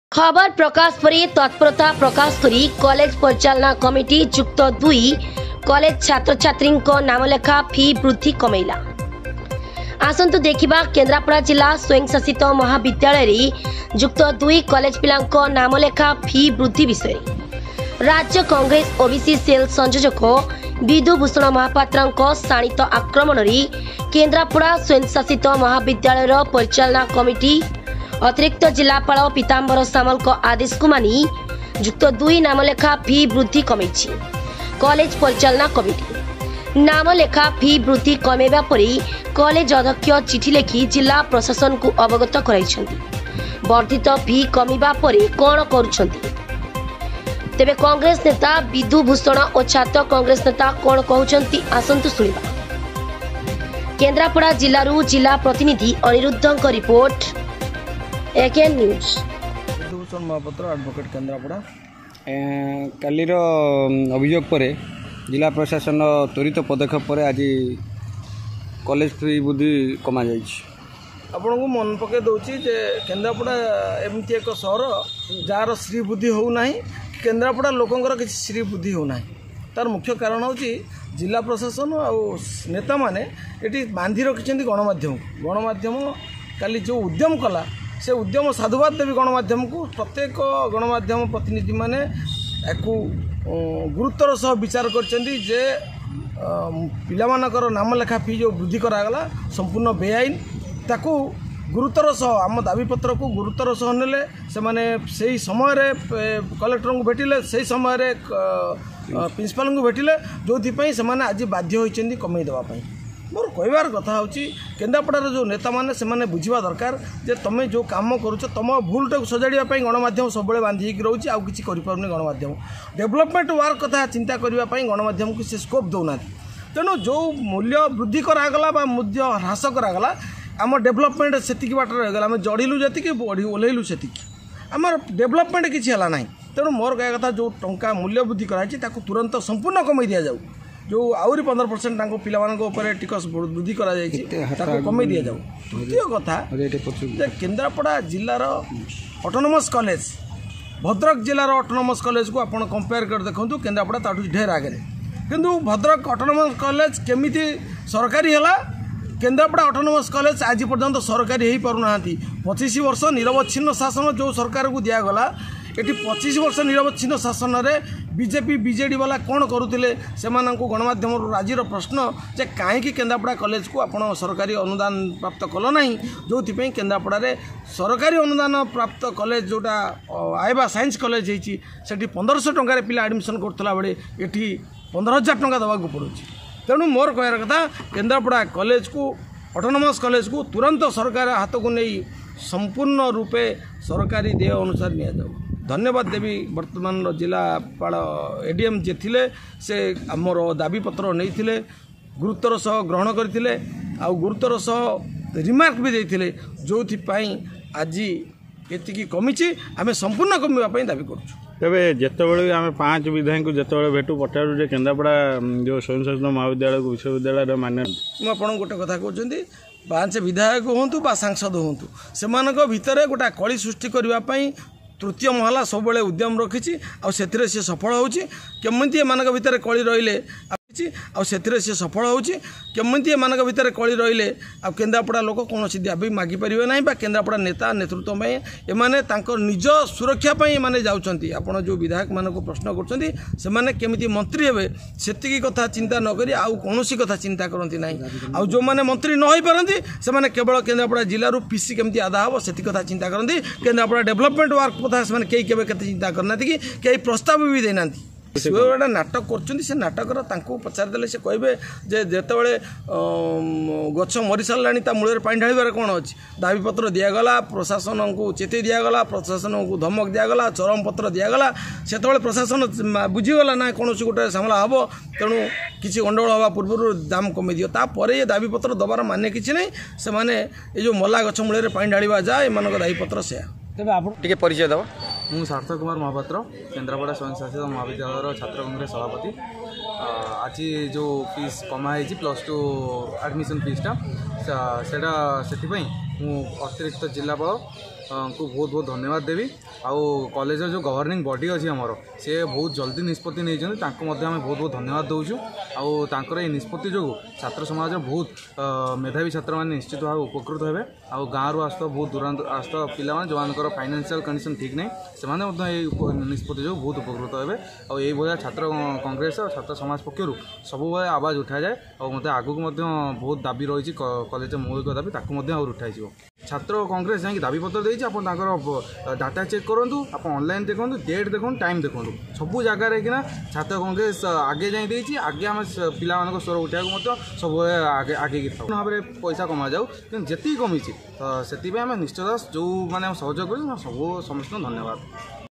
କଲେଜ ପରିଚାଳନା କମିଟି +୨ କଲେଜ ଛାତ୍ର ଓ ଛାତ୍ରୀଙ୍କ ନାମଲେଖା ଫି ବୃଦ୍ଧି କମେଇଲା ll AKN NEWS
ଖବର ପ୍ରକାଶ ପରେ ତତ୍ପରତା ପ୍ରକାଶକରି କଲେଜ ପରିଚାଳନା କମିଟି +୨ କଲେଜ ଛାତ୍ର ଓ ଛାତ୍ରୀଙ୍କ ନାମଲେଖା ଫି ବୃଦ୍ଧି କମେଇଲା ll AKN NEWS